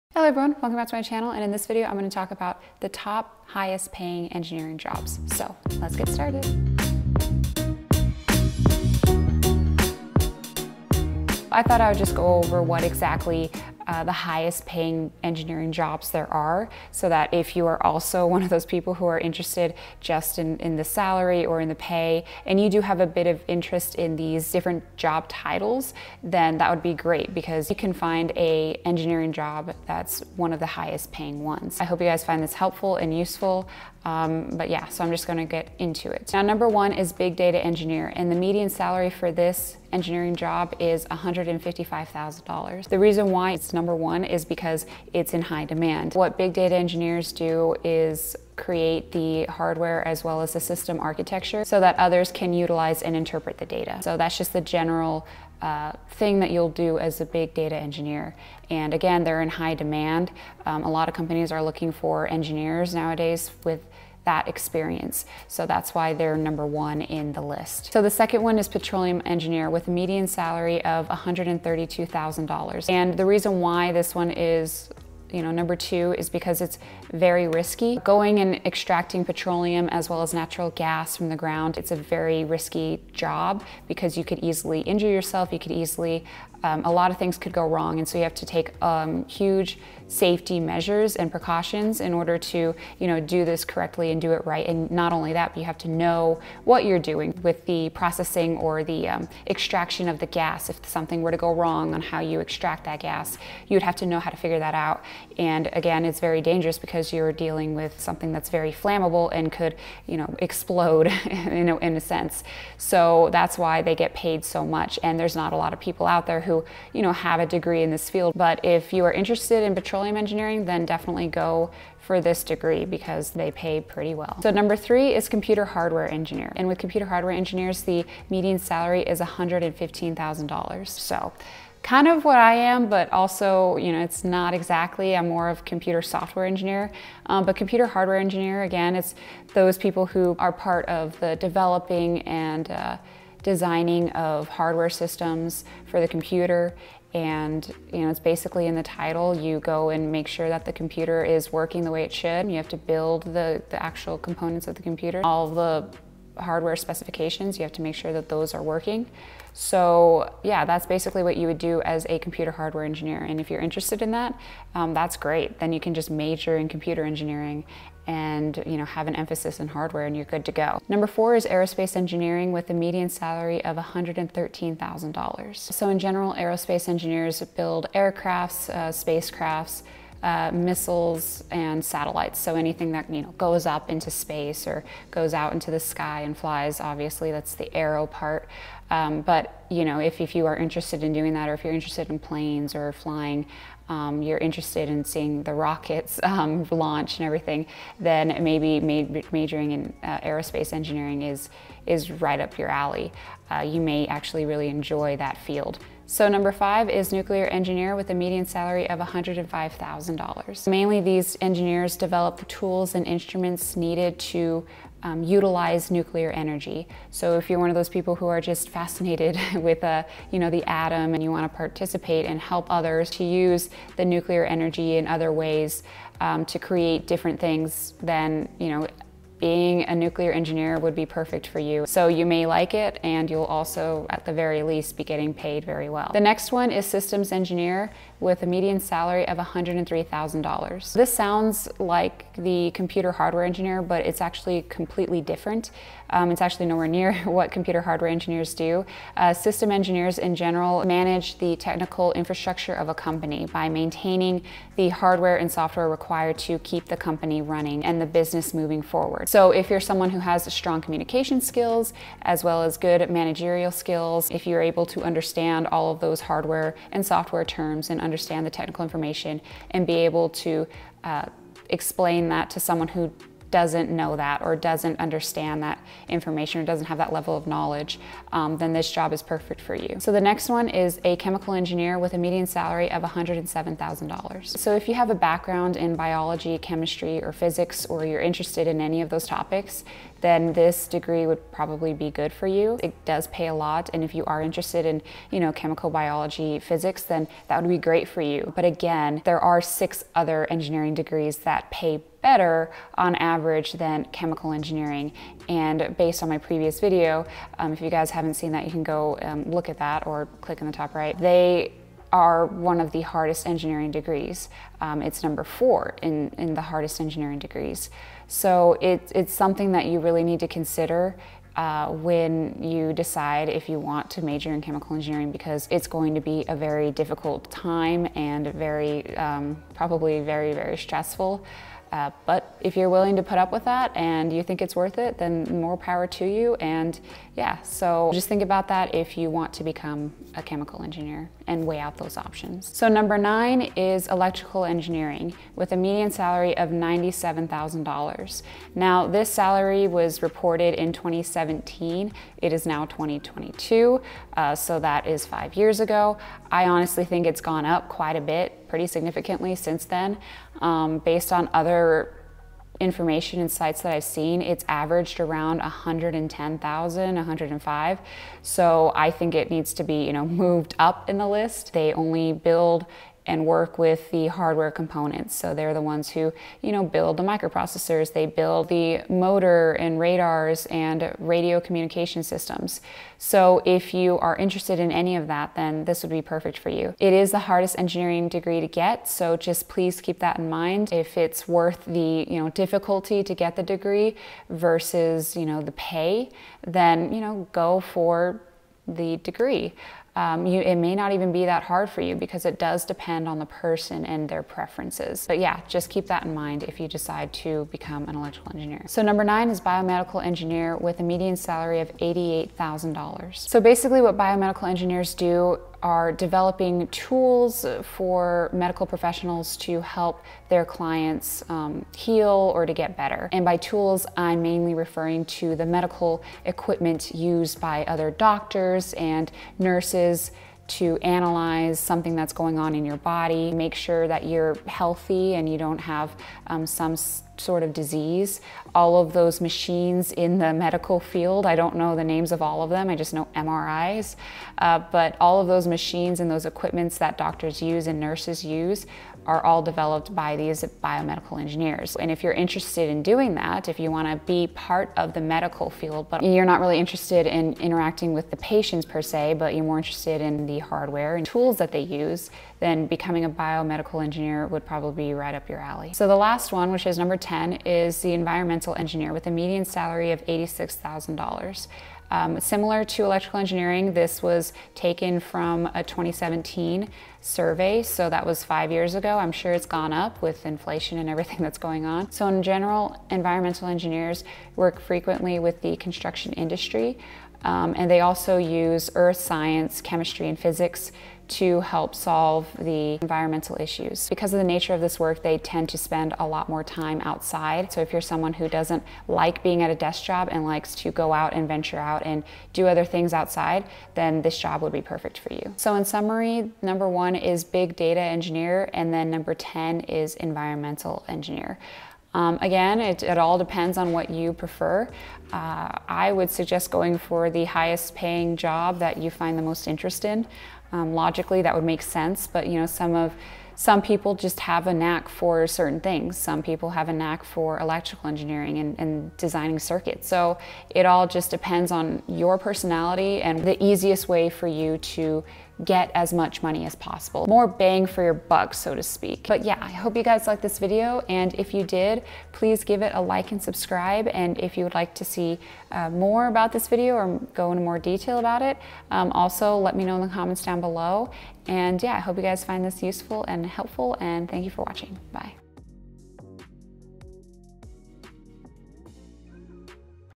TOP 10 HIGHEST PAID ENGINEERS | Median Salary | What I Know
Hello Everyone!

A bit late on the upload this week but we still got it done! This video is pretty self explanatory - We are going to go over the top 10 highest paying engineering job fields in the US. They are ranked on the median salary and some have been recorded from 2017 so the actual rankings may vary for 2022.

But if you happen to be interested in any of these fields then I'd suggest definitely majoring in one of these fields (interest in work + high salary = life will be good).